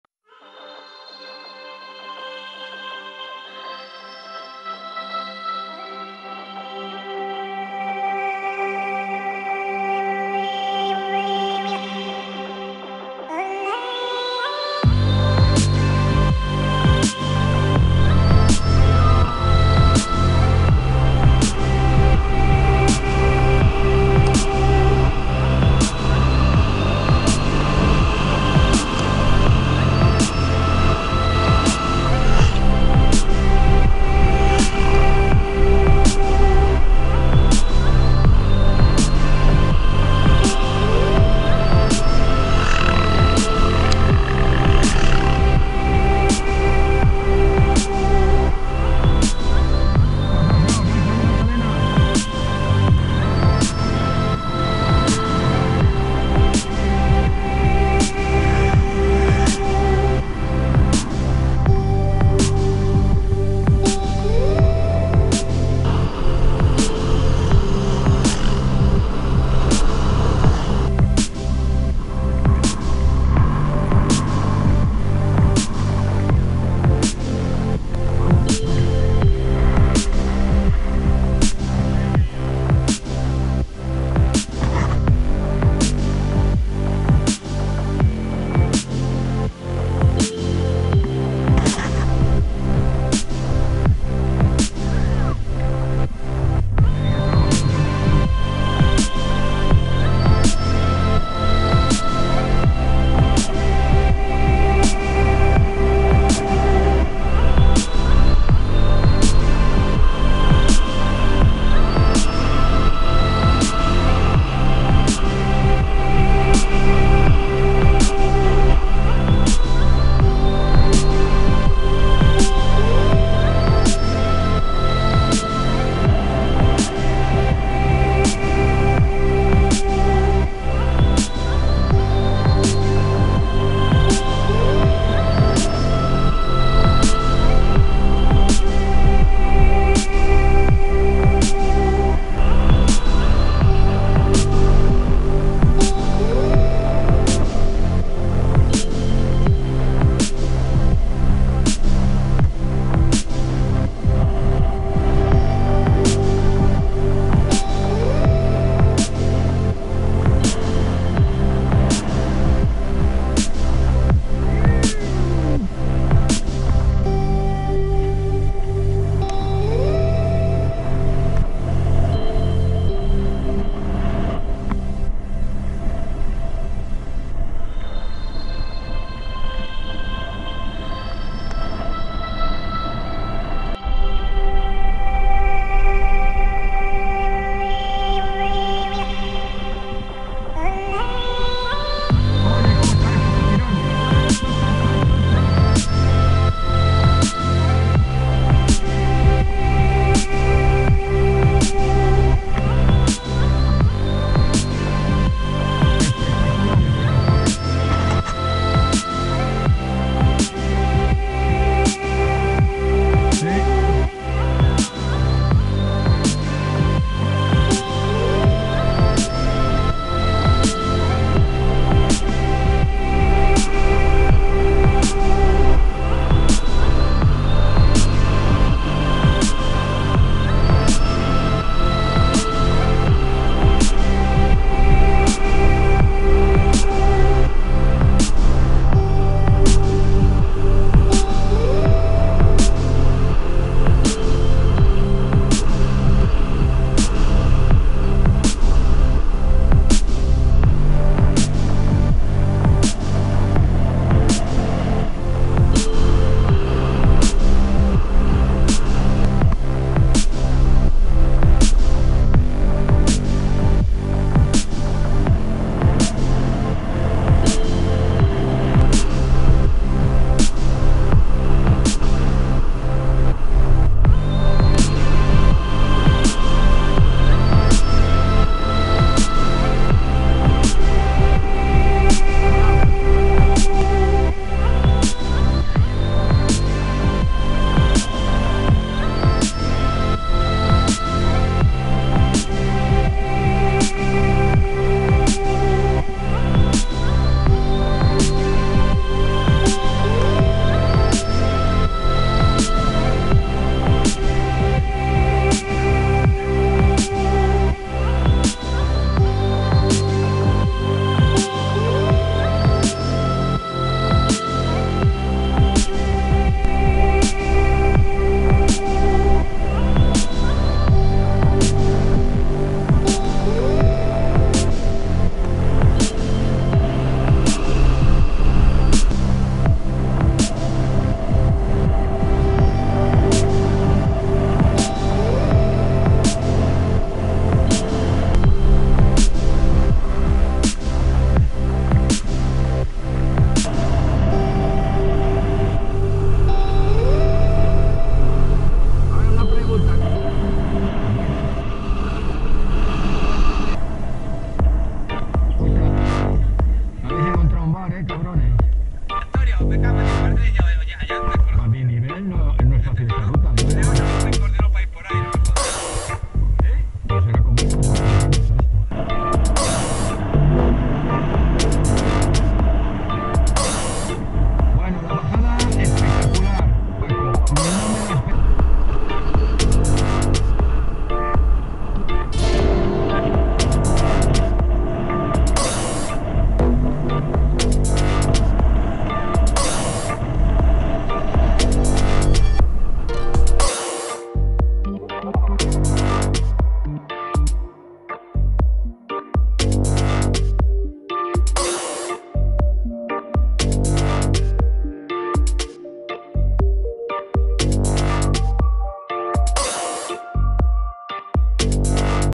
BTT Bajada Calar Alto 2019. Amigos de la MTB Valladolid.
Deporte con la bici de Montaña: Después de subir al Calar Alto, desde Baza, bajamos todos por una pista de 37 km,  hasta llegar al pueblo de  Serón, y regresar de nuevo a Baza. En total fueron más de 110 km de ruta.